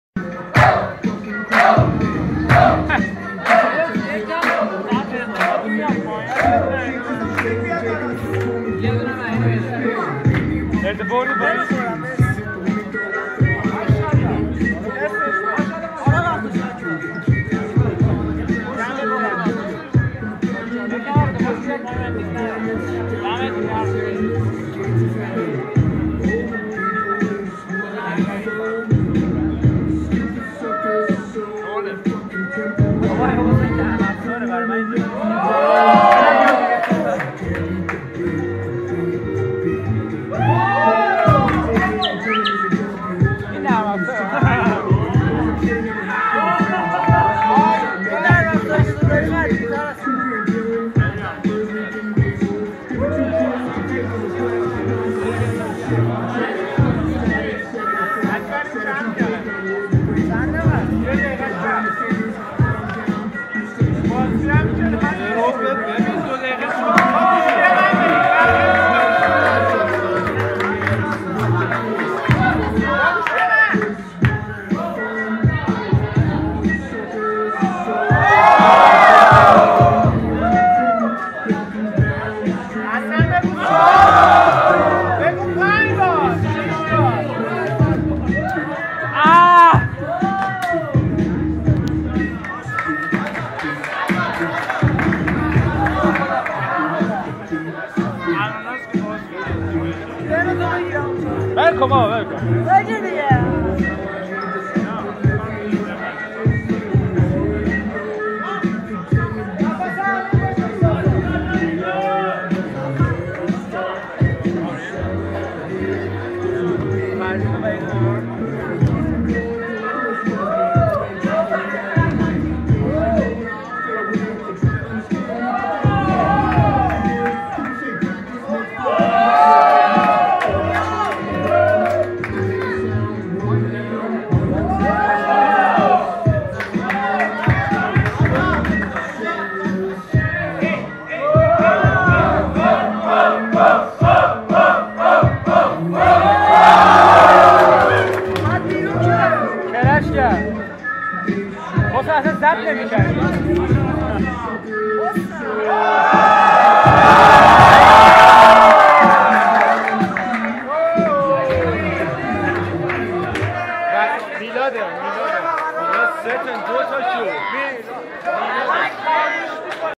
Controversial battle between Bboy Commando (Rahaa Crew) and Bboy Sasan.flv
This is the video of controversial battle between Bboy Commando (Rahaa Crew) and Bboy Sasan, after this two rounds all five judges gave tie break!
Bboy Commando is in right side the screen and started the battle and Bboy Sasan is in left side, watch this video and leave your comment down here or on Bboy Commando's page, peace.